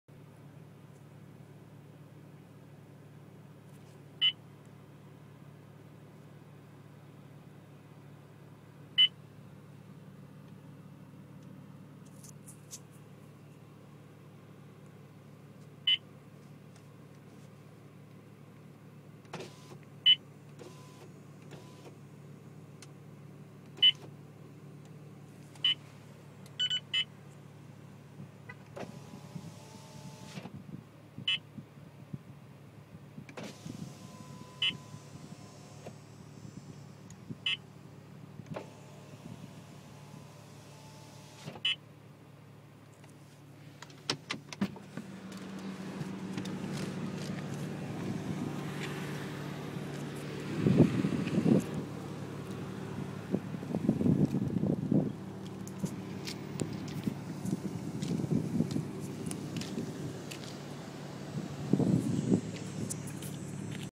5% tint Blacked out BMW 328i !
5% suntek all the way around, with a strip.

blacked out bmw 328i dark!